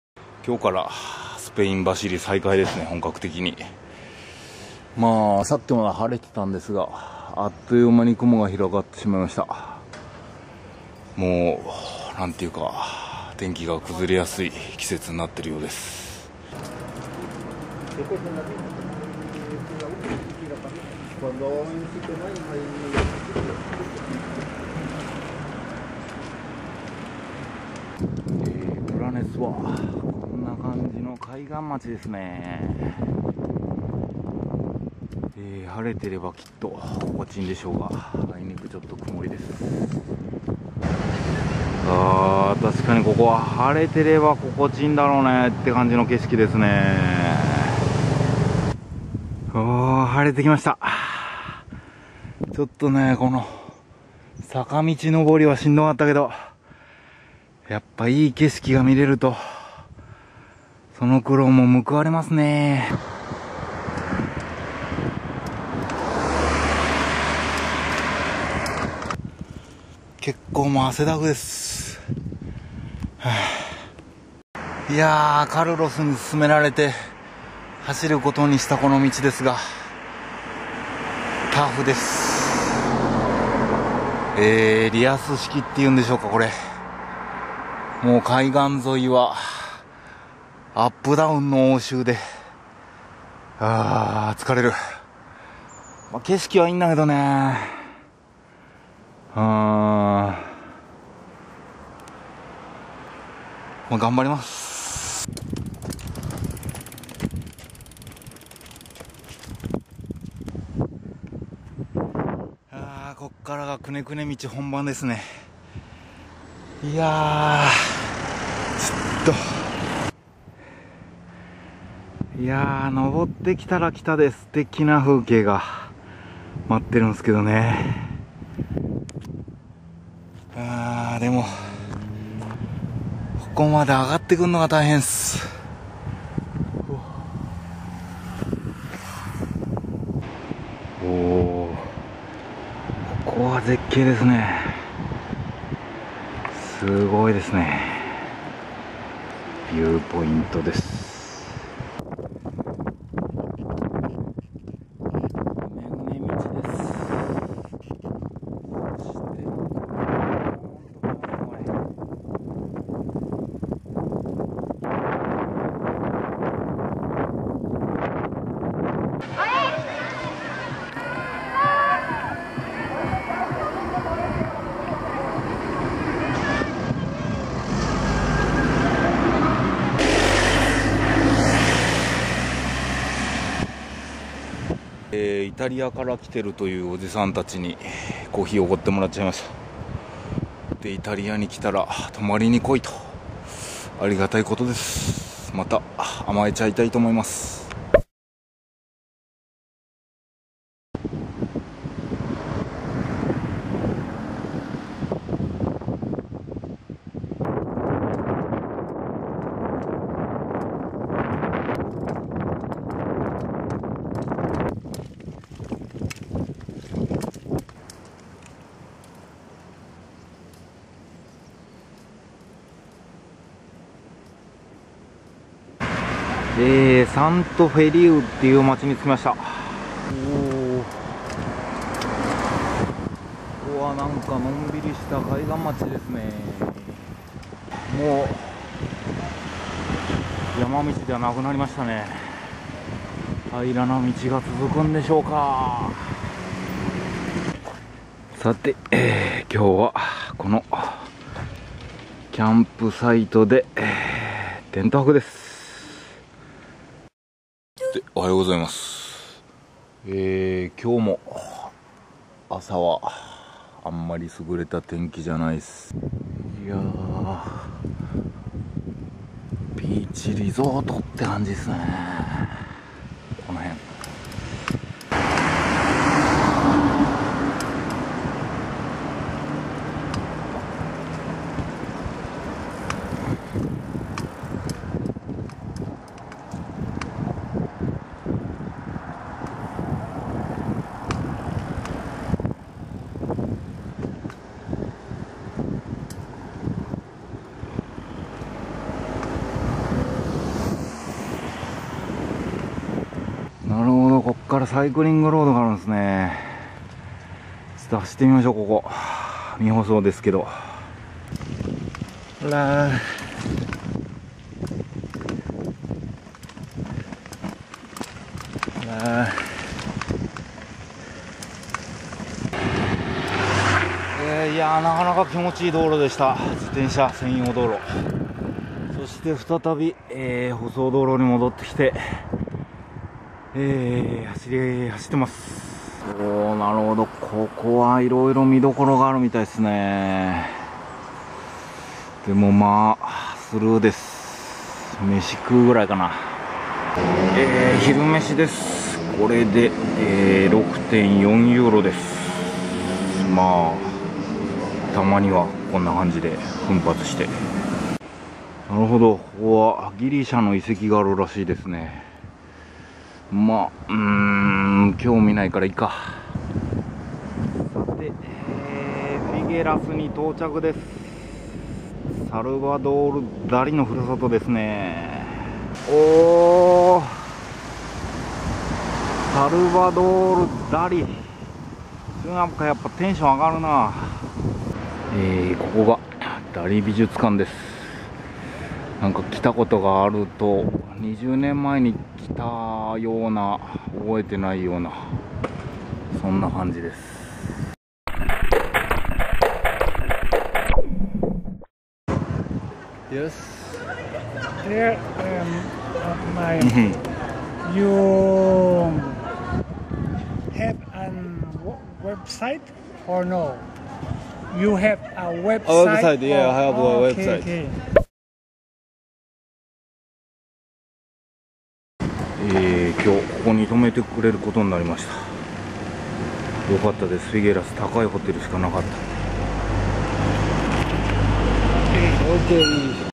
FOTP vol.251 : Sea side road cycling
スペインの北部を走るのなら、ぜひ、ブラネスの先にあるヨレット・デ・マルというビーチ町からの海岸沿いの道を走るといい、と、カルロスからアドバイスをもらって走り始めた海岸道。ちょうどヨレット・デ・マルという町を過ぎたあたりから、天気が回復して、青空が覗き始めた。おお、やっぱり青空の海岸道は心地いい、と思ったのもつかの間。ヨレット・デ・マルを過ぎたところから、道がグネグネでアップダウンがめっちゃ激しく・・・バテバテ。久々のチャリ走りには、厳しすぎる道だよぉ。